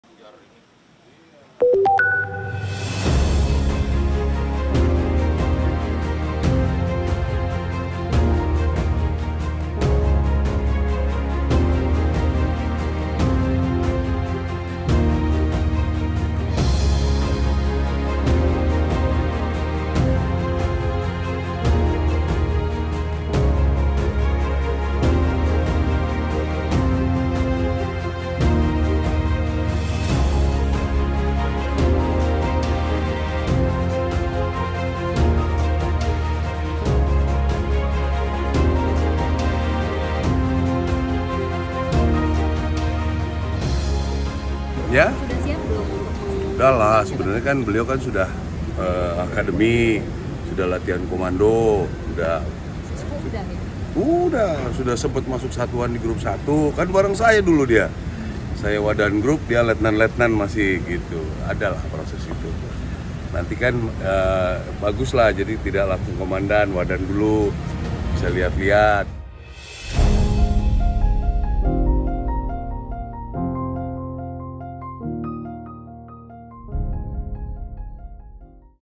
Ini kata KSAD Maruli Simanjuntak terkait promosi jabatan Mayor Teddy
ANTARA - Kepala Staf TNI Angkatan Darat, Jenderal TNI Maruli Simanjuntak buka suara soal rotasi jabatan Mayor Teddy sebagai Wakil Komandan Batalyon Infanteri Para Raider 328/Dirgahayu. Saat dijumpai di Istana Kepresidenan Jakarta, Rabu (13/3), KSAD mengatakan Mayor Teddy memiliki rekam jejak yang layak menempatkannya di posisi baru tersebut.
(Aria Cindyara/Yogi Rachman/Andi Bagasela/Farah Khadija)